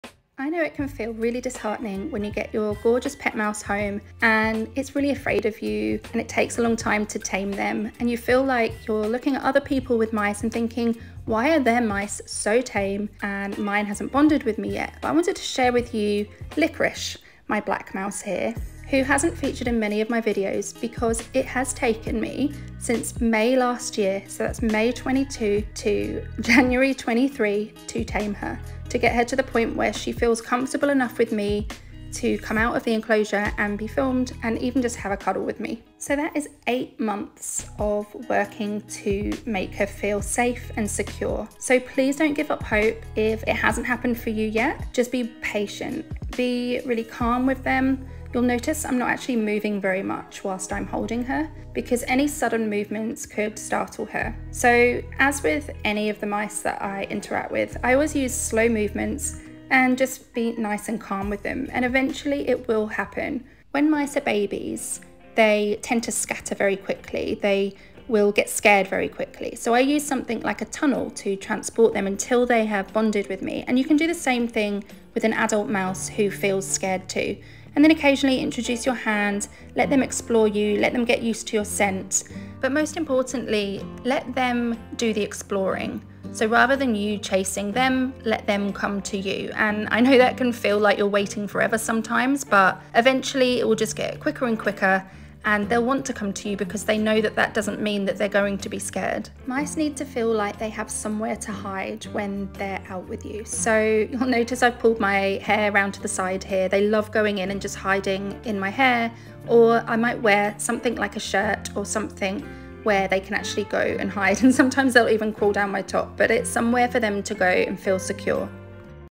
This is what it’s like taming pet mice
Let's talk about taming pet mice. Things aren't always as they seem! I share a lot of videos on how to tame pet mice. In this video though, I share the other side of bonding with mice, when things feel a little difficult.

They are incredibly loving, but with each mouse having its own unique personality, sometimes, it takes time for them to build trust and get mice to like you.